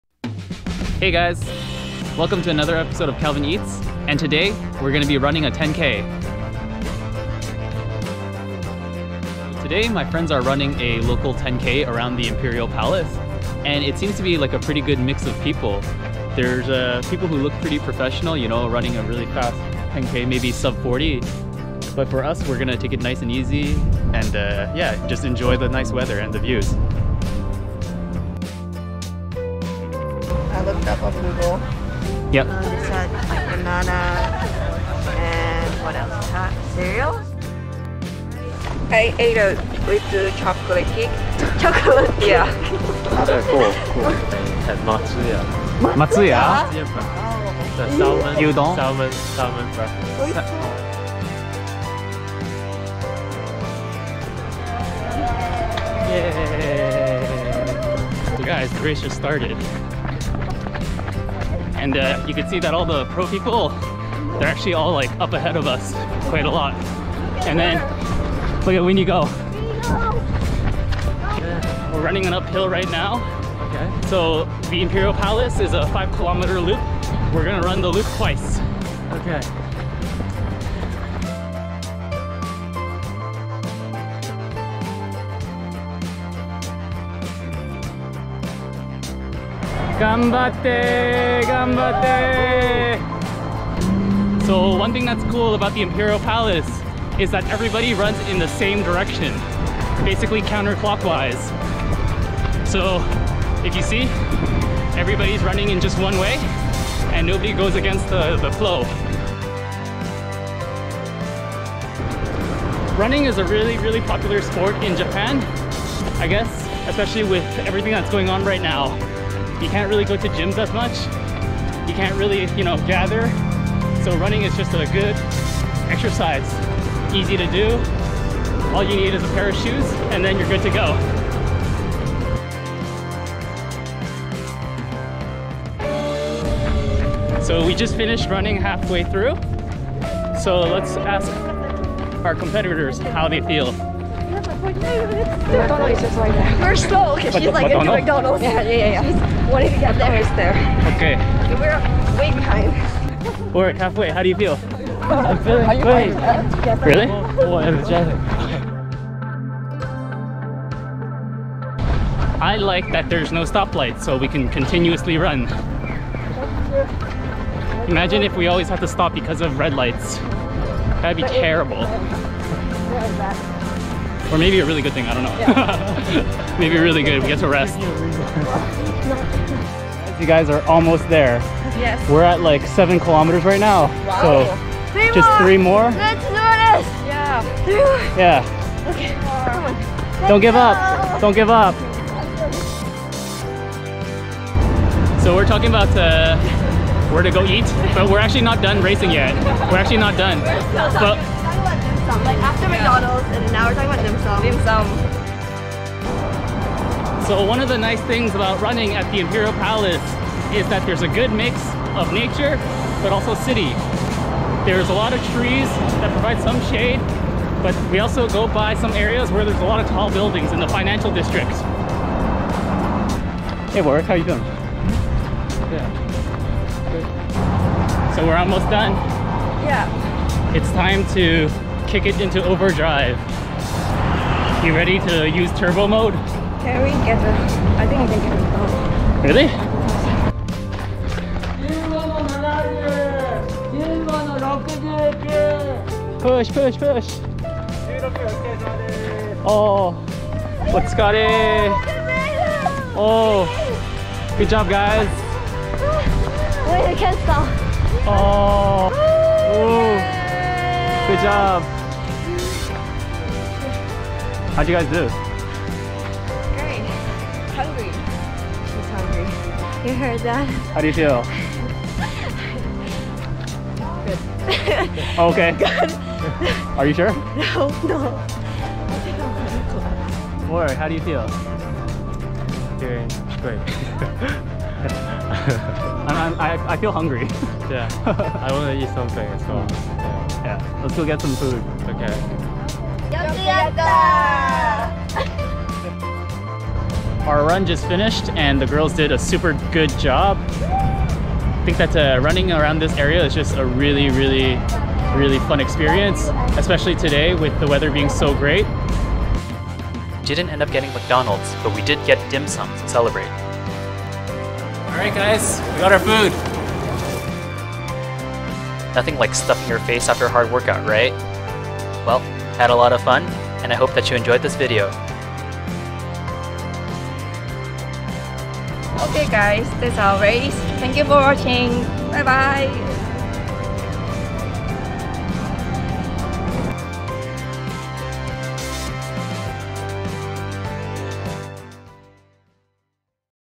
Running in Tokyo Special - Imperial Palace 10K
Special Running in Tokyo Video - My friends joined a 10KM fun run around the imperial palace. Join us on this adventure!